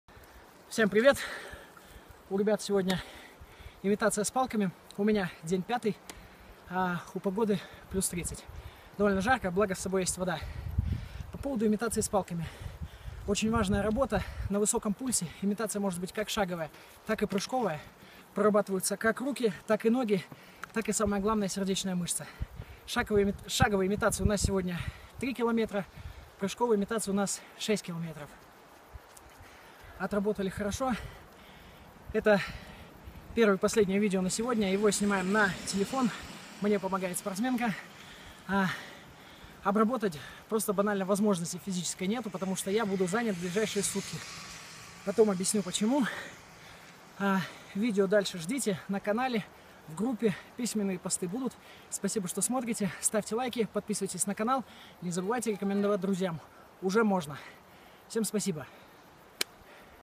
День 5. Имитация с палками.
Сегодня сделали две тренировки.

1 тренировка (после которой снято видео).

3 км шаговая имитация (парень на заднем плане шагал начало подъёма)
6 км прыжковая имитация (в середине подъёма он перешёл именно на прыжковую)
Потом - ОФП и статика (её теперь делаем ещё больше)
Поработали на славу, добрав все чего не хватило вчера.
Работа на пульсе сильно выше среднего, да ещё и в горку, да на жаре - это хорошо скажется на общей подготовки. В общем, доволен.

2 тренировка ( на видео не попала ).
Кросс в свободном темпе - 8 км.
По пути заскочили на ручей, охладили лица, а кто то - и полностью окунулся. Не, ну а что вы хотели? Жара!
Потом много растяжки с акцентом на ноги.
Потом немного поиграл на турнике с ребятами в "лесенку".
А после - похватали велосипеды и поехали окунуться на реку. Красота!

Общими итогами дня доволен как слон и в очередной раз утверждаюсь в мысли что жизнь - прекрасна. А ещё она v2.0!:)

P.S. Сейчас для жителей больших городов это наверное звучало дико...тренировка...ручей...река...
Но да! Это один из огромных плюсов жизни в маленьком городишке!